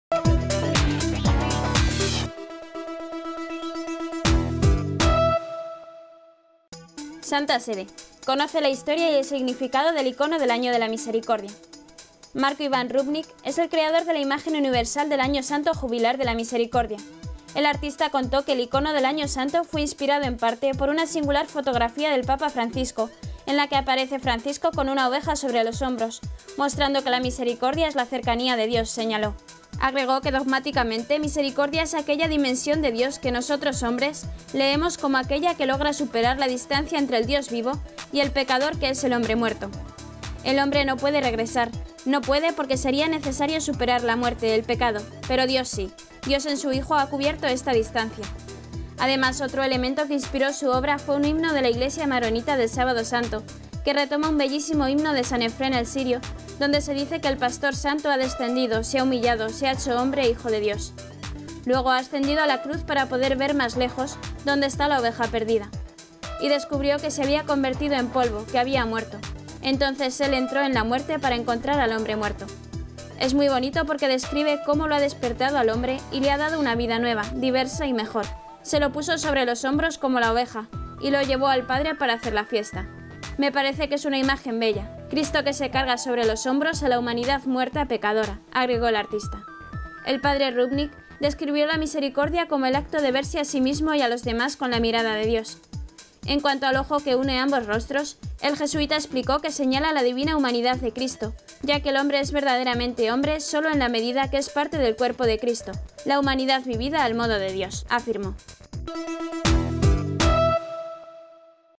Vídeo Noticia: Conoce la historia y el significado del ícono del Año de la Misericordia
Marko Ivan Rupnik es el creador de la imagen universal del Año Santo Jubilar de la Misericordia. El artista contó que el ícono del Año Santo fue inspirado, en parte, por una singular fotografía del Papa Francisco, en la que aparece Francisco con una oveja sobre los hombros” mostrando que “la misericordia es la cercanía de Dios”, señaló.